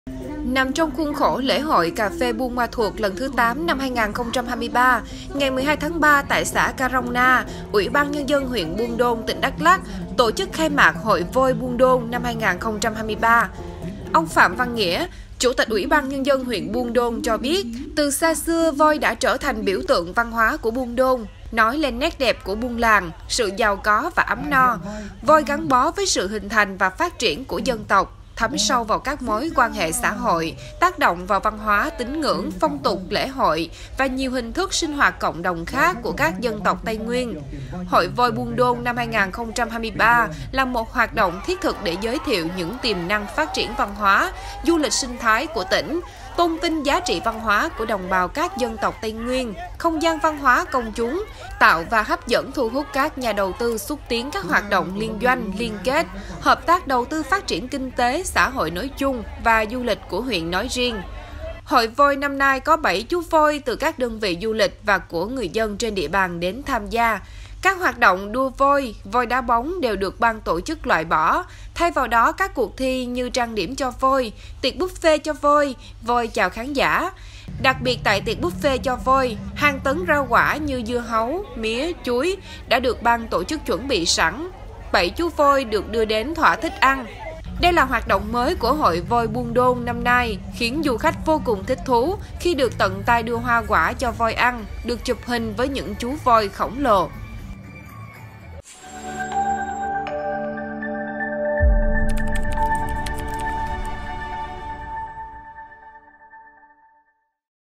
7 chú voi Buôn Đôn thưởng thức đại tiệc buffet với hàng tấn rau quả
Nằm trong khuôn khổ Lễ hội cà phê Buôn Ma Thuột lần thứ 8 năm 2023, ngày 12-3, tại xã Krông Na, UBND huyện Buôn Đôn (tỉnh Đắk Lắk) tổ chức khai mạc Hội voi Buôn Đôn năm 2023. Đặc biệt tại tiệc buffet cho voi, hàng tấn hoa quả như: Dưa hấu, mía, chuối... đã được Ban tổ chức chuẩn bị sẵn, 7 chú voi được đưa đến thỏa thích ăn.